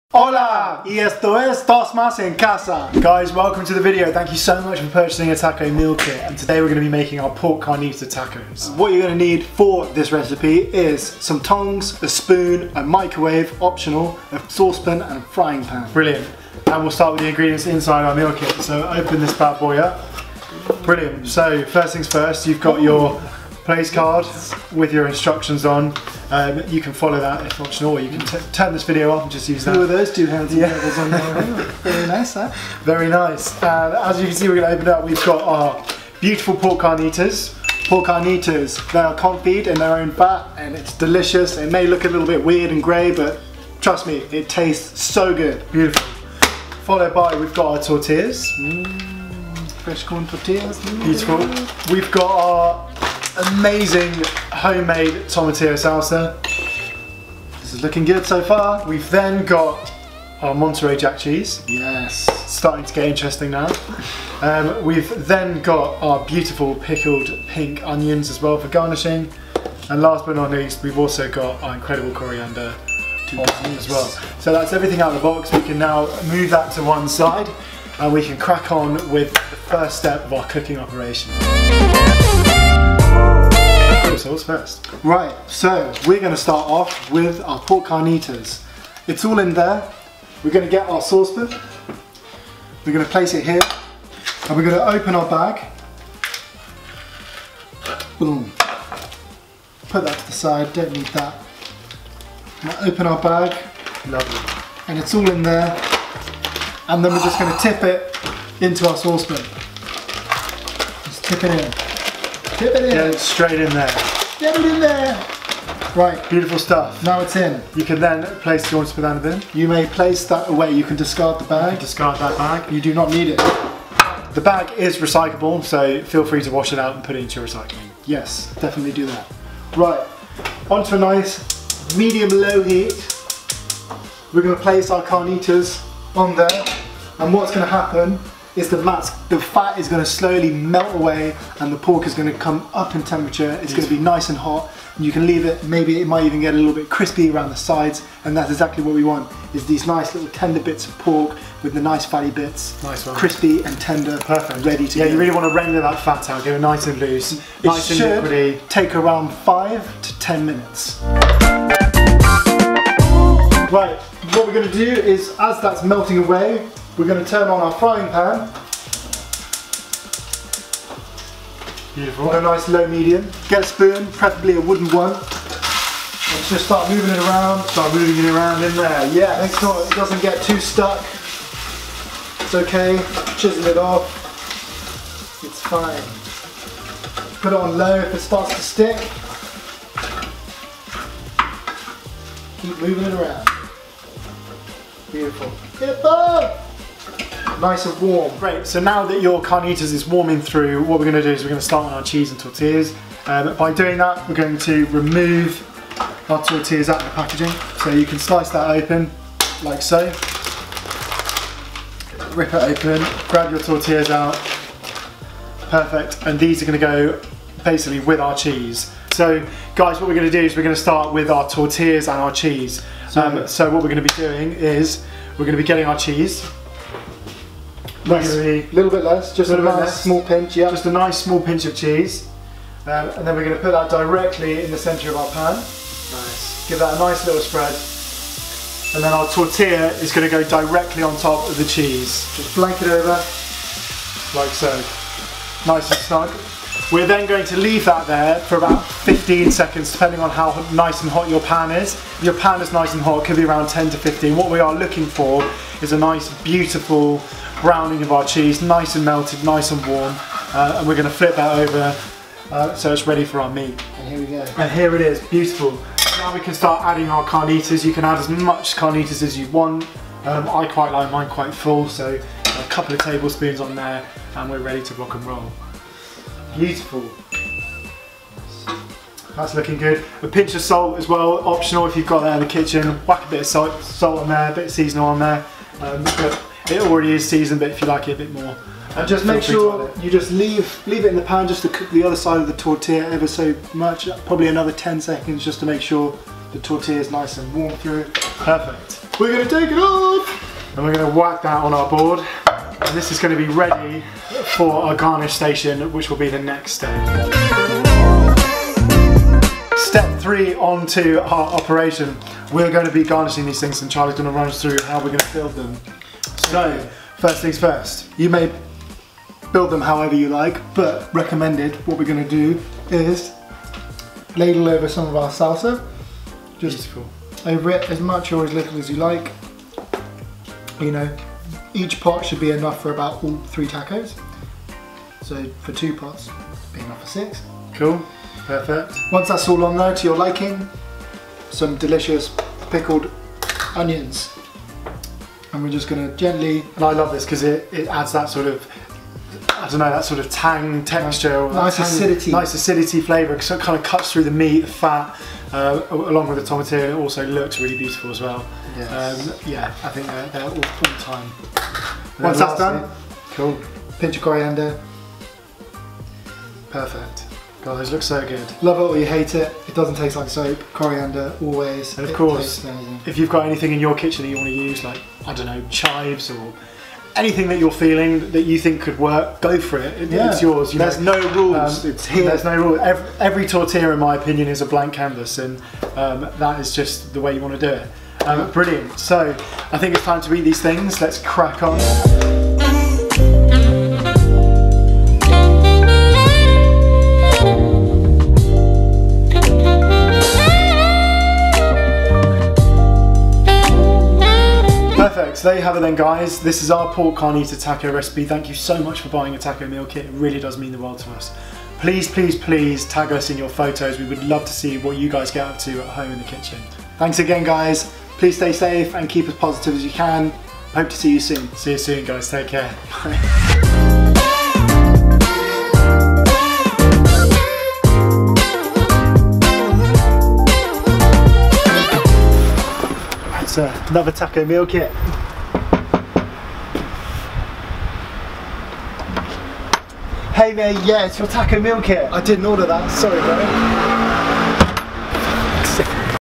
DOS MAS TACO MEAL KIT - PORK CARNITAS
This is how to make our Pork Carntia Taco at home! This is the exact recipe we use at our locations and when serving on the streets of London. This is by far our customer's favourite taco!